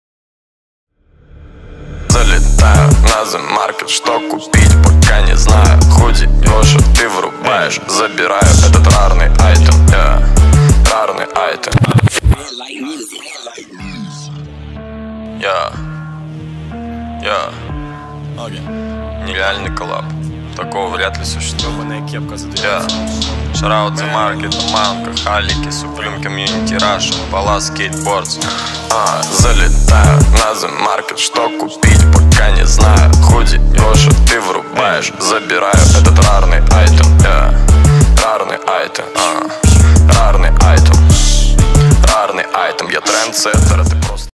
AKRO JumP
Ребят это наше новое клановое видео.в этом видео учавствовали: Руслан Глебов. Данил Иванников(создатель клана). Антон Несмеянов и Кирилл Усанов.